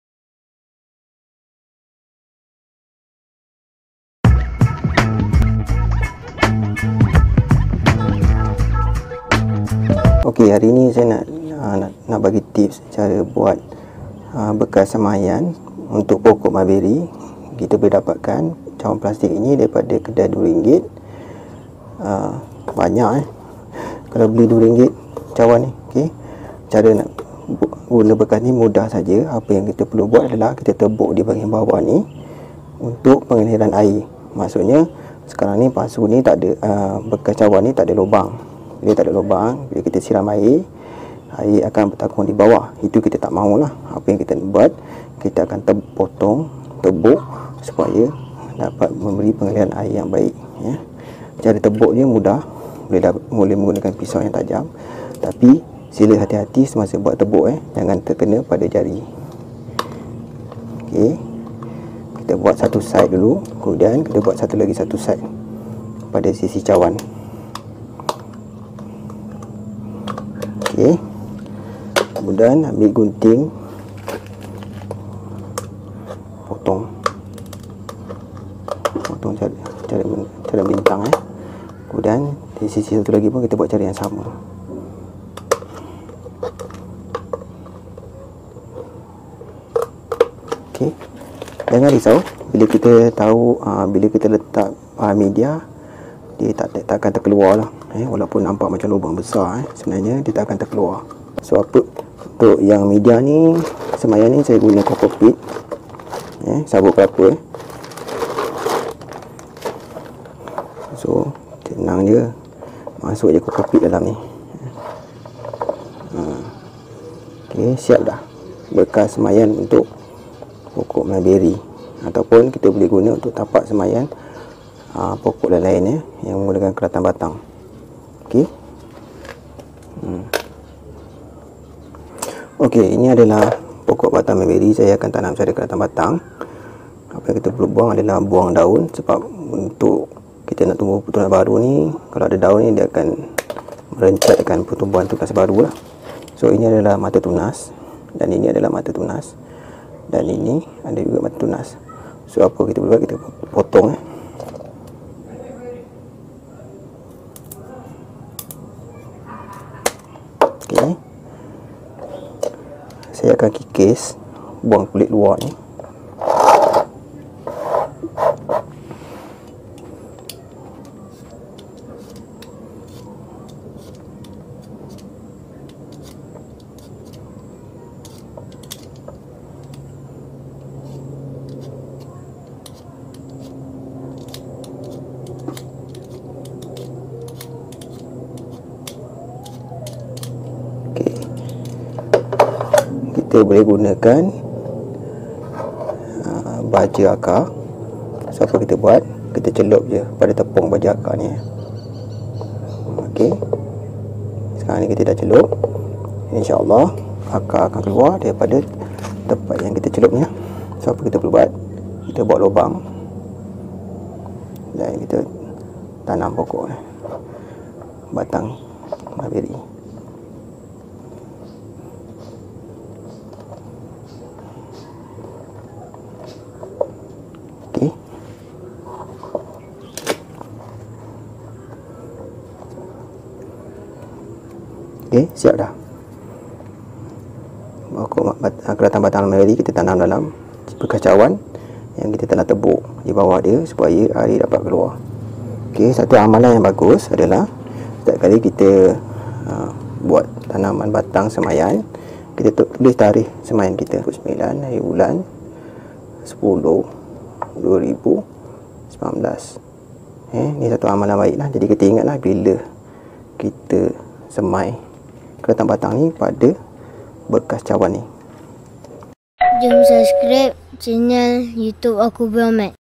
Semai keratan batang guna Cocopeat atau serbuk Sabut Kelapa
Cara mudah nak semai keratan batang boleh guna bekas cawan plastik sebagai bekas semaian.

Mengunakan Cocopeat atau serbuk Sabut Kelapa sebagai media semaian mengantikan tanah.

Baja Organik Milagro atasi masalah pokok yang segan untuk berbunga dan berbuah.

Perkakas rakaman yang digunakan untuk rakaman.